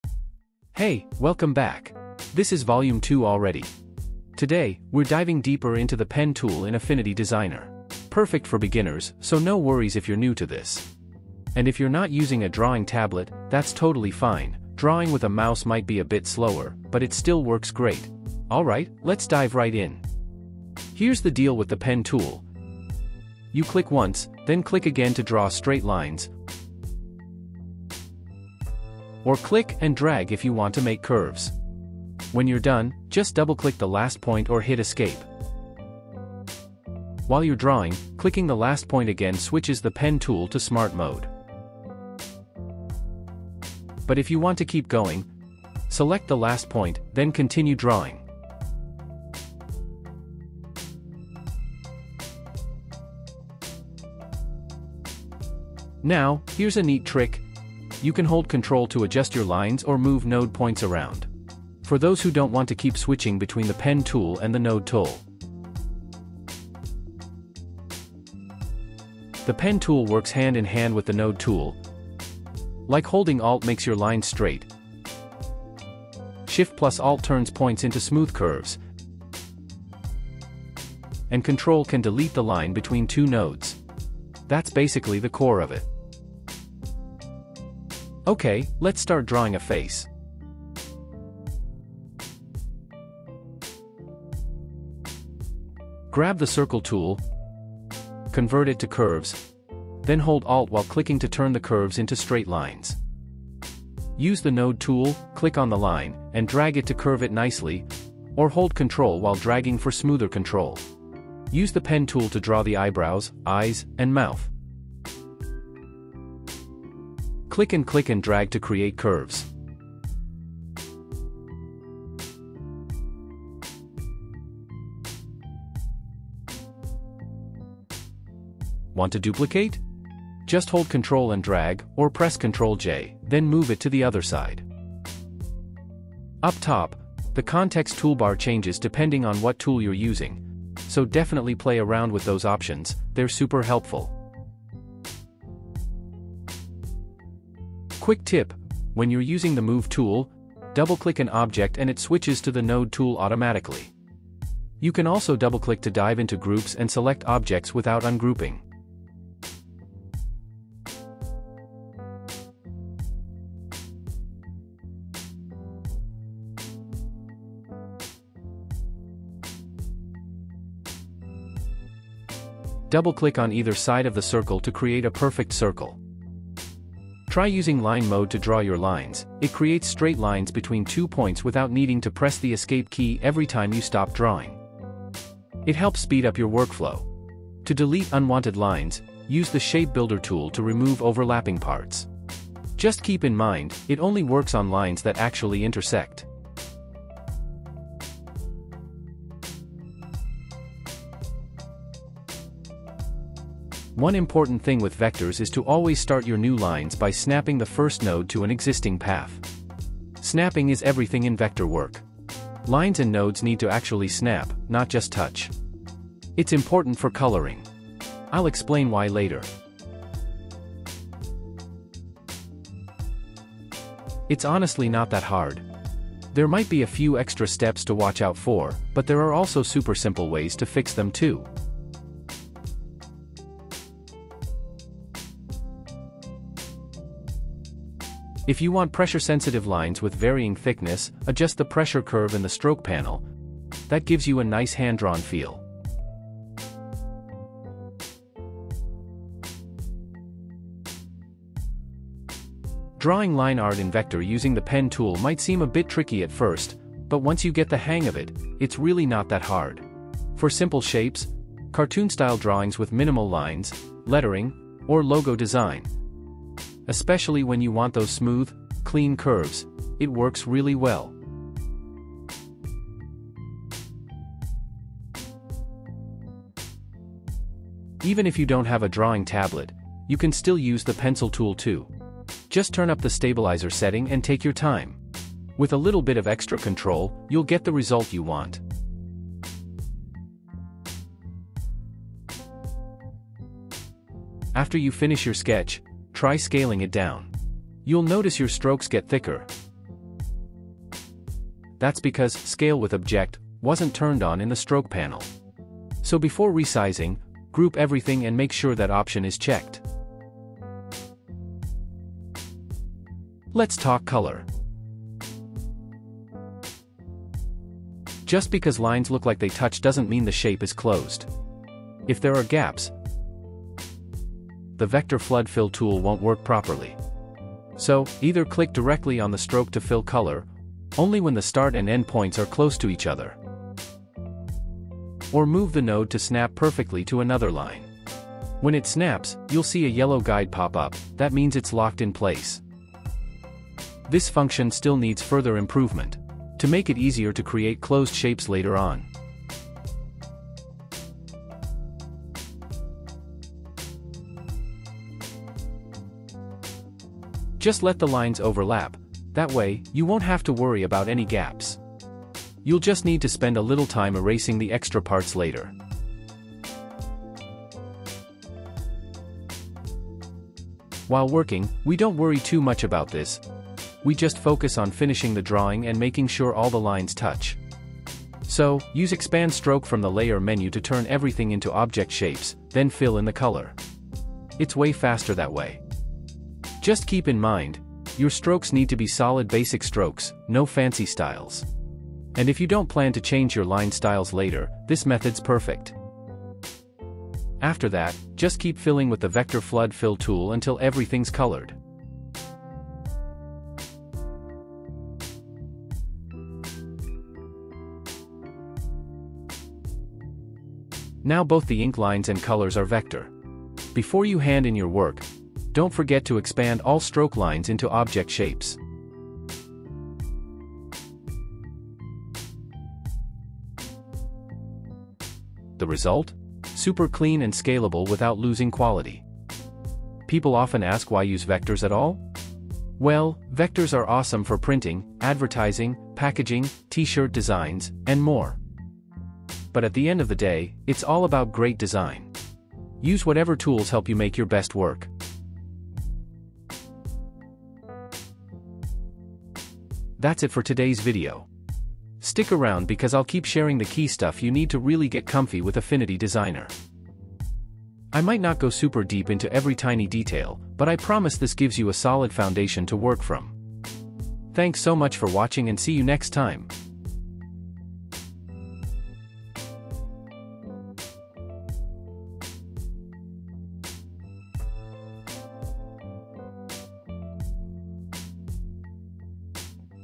Affinity Designer For Beginners : The Pen Tool
V0lume 2 is here! Let's dive into the Pen Tool in Affinity Designer-- perfect for beginners and super useful even if you're just using the mouse. Packed with simple tips to help you draw, color, and get clean Vector results fast. Hope you enjoy and like it.